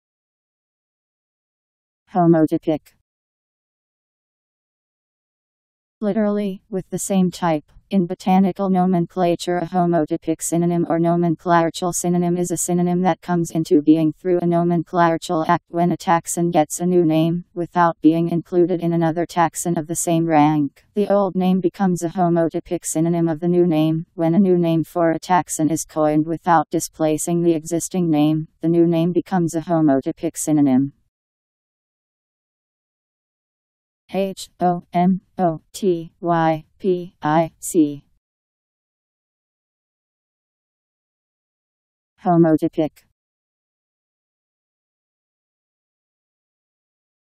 Homotypic Meaning
Video shows what homotypic means. literally "with the same type". In botanical nomenclature a homotypic synonym (or nomenclatural synonym) is a synonym that comes into being through a nomenclatural act
when a taxon gets a new name, without being included in another taxon (of the same rank). The old name becomes a homotypic synonym of the new name.
when a new name for a taxon is coined without displacing the existing name, the new name becomes a homotypic synonym..  Homotypic Meaning. How to pronounce, definition audio dictionary. How to say homotypic. Powered by MaryTTS, Wiktionary